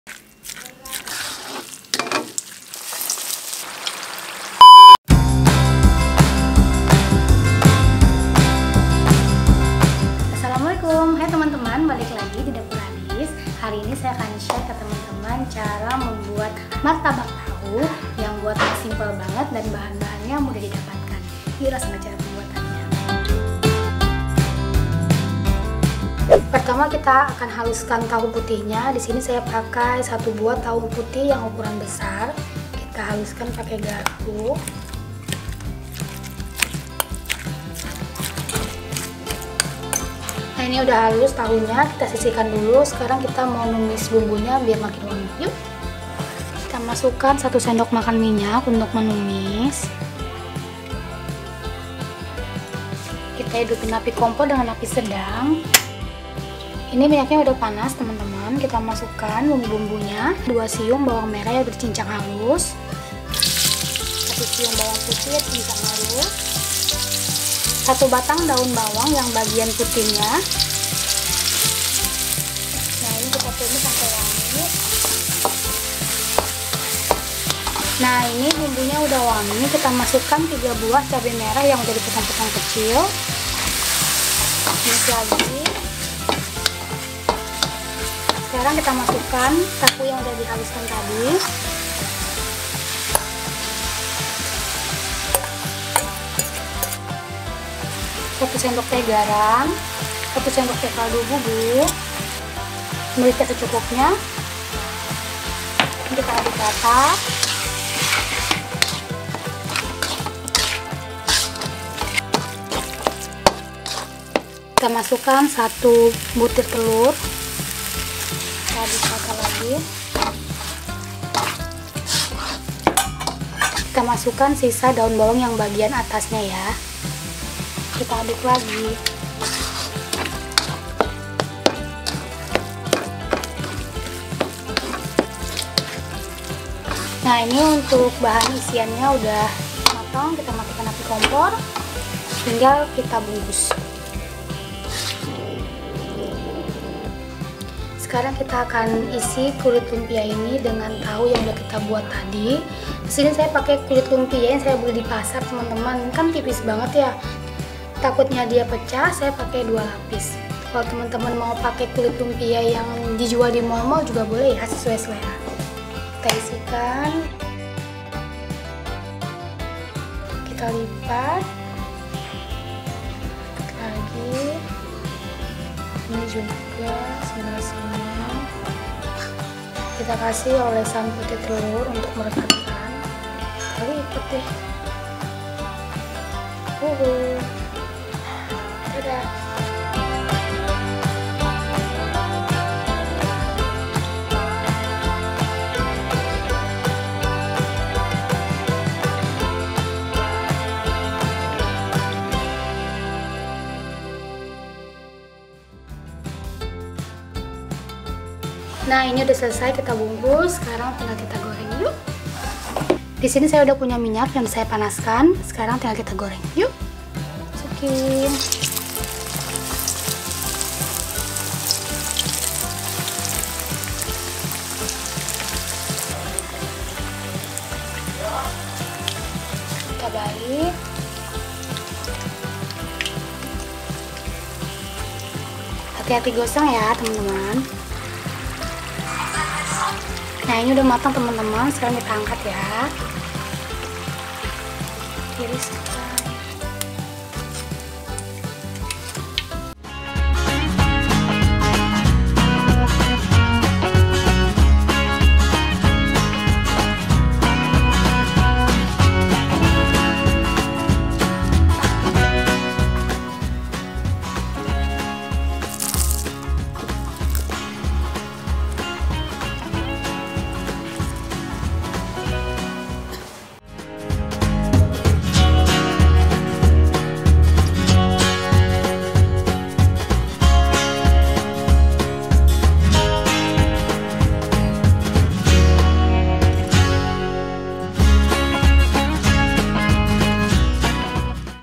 RESEP MARTABAK TAHU ALA DAPUR ADIS
- 1 buah tahu putih besar
- 2 siung bawang merah
- 1 siung bawang putih
- 1 batang daun bawang
- 3 buah cabe merah keriting
- 1 sdt garam
- 1 sdt kaldu bubuk
- Merica bubuk secukupnya
- 1 butir telur
- Kulit lumpia secukupnya
- Minyak goreng untuk menggoreng dan menumis

Cara membuat martabak tahu :
1. Haluskan tahu
2. Tumis bawang merah, bawang putih, batang daun bawang hingga wangi
3. Masukkan cabe merah keriting, tumis kembali hingga wangi
4. Masukkan tahu yang sudah dihaluskan
5. Tambahkan garam, kaldu bubuk, merica bubuk. Lalu aduk merata
6. Tambahkan telur, aduk merata masak hingga matang
7. Tambahkan daun bawang, aduk merata
8. Letakkan tahu tumis keatas kulit lumpia lipat, lalu beri olesan putih telur untuk merekatkan
9. Setelah semua dilipat, panaskan minyak lalu goreng martabak tahu hingga kuning keemasan. Hati-hati gosong :D
10. Setelah matang, angkat dan tiriskan.
11. Martabak tahu dapat dihidangkan selagi hangat untuk hidangan berbuka puasa maupun untuk cemilan keluarga

Sampai jumpa di resep dapur adis selanjutnya. :)

Jangan lupa tag dan share ke instagram @adistinurarifanillah atau @dapuradis kalau sudah recook resep ini 😊